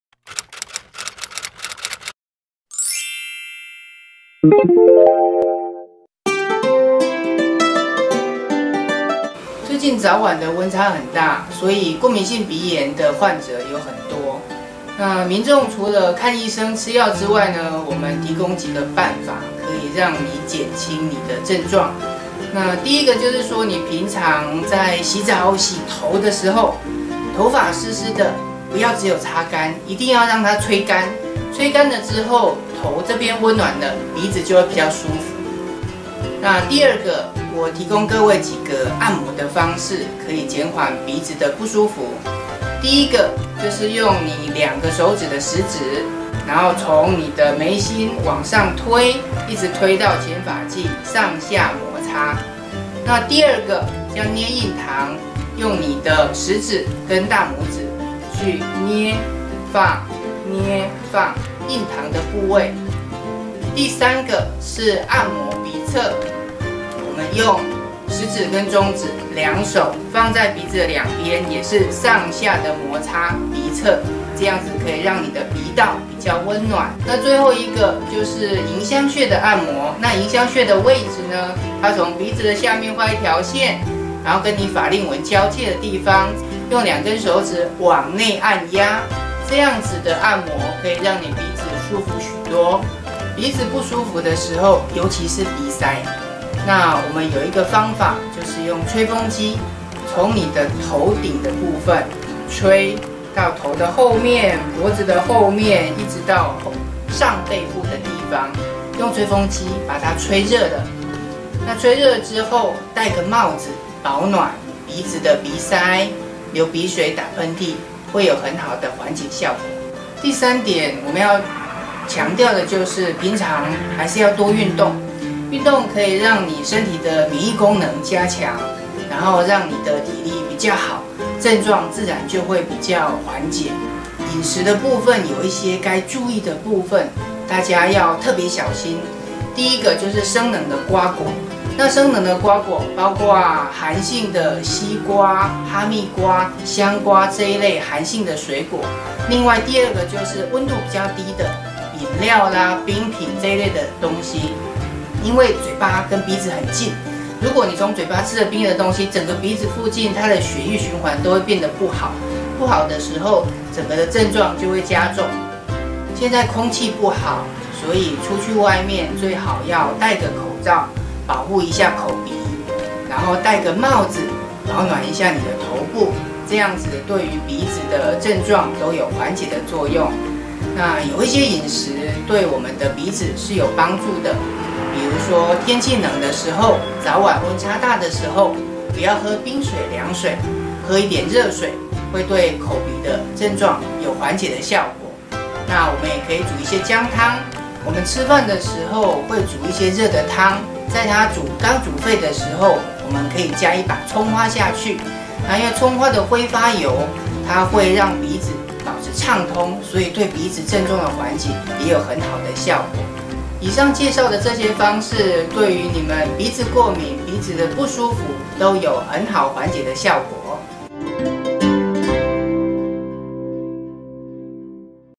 高堂中醫-謝志信醫師-鼻子過敏怎麼辦？
根據統計，台灣人每五人就有一人有過敏性鼻炎，特別在寒冷的時候更是發作的時候，尤其台灣屬潮濕氣候，現在又有嚴重的空污問題，讓鼻病更嚴重，那麼，我們就來聽聽謝志信醫師告訴您中醫有哪些妙招可以緩解鼻病。